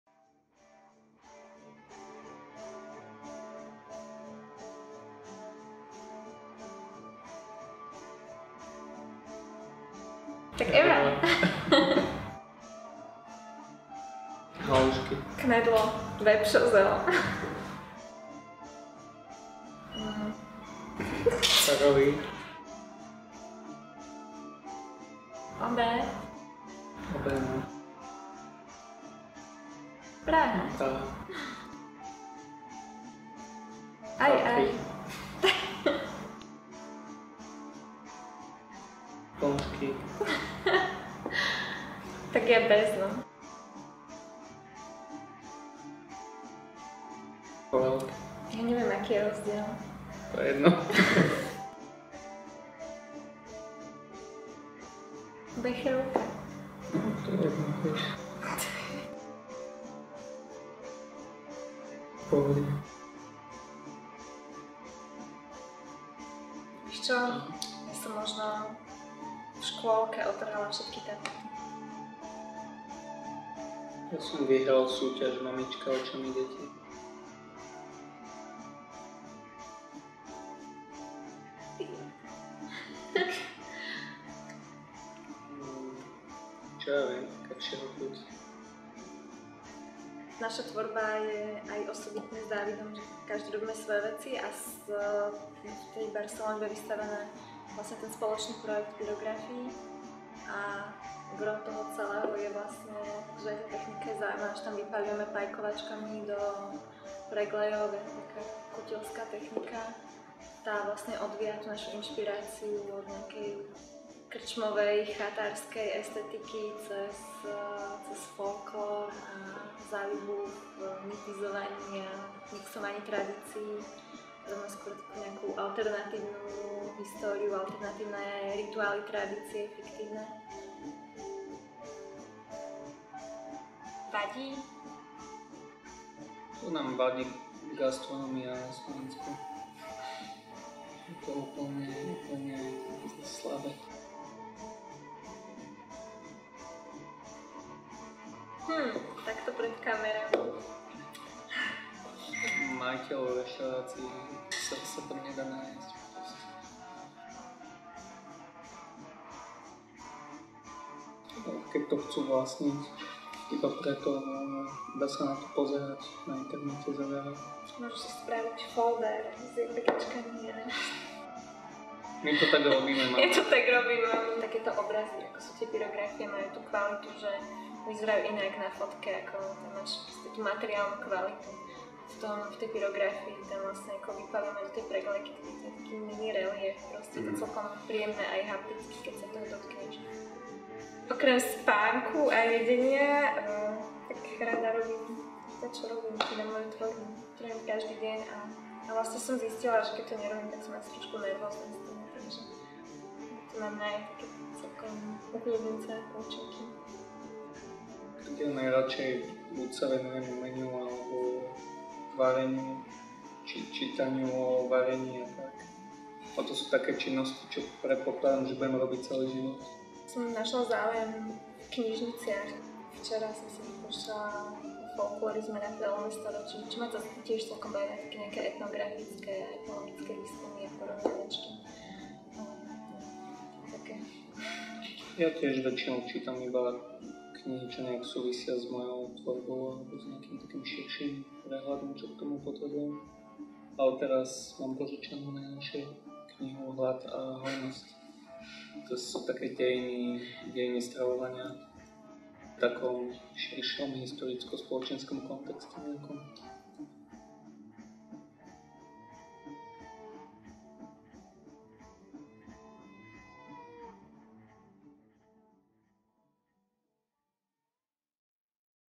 PROJECT CZECH SLOVAK interview with Jarmila Mitriková and David Demjanovič
Jarmila Mitríková and David Demjanovič, this author duo has been active on the Slovak
artistic scene for only a short time, but they have
already attracted attention with their specific manuscript
characterised above all by their technique
of burning into wood - pyrography. The technique,
which also appears frequently in expressions of folk
art, often forms the thematic basis for many of their
works. This is reflected especially in references to
folk literature, folklore, religion and national identity.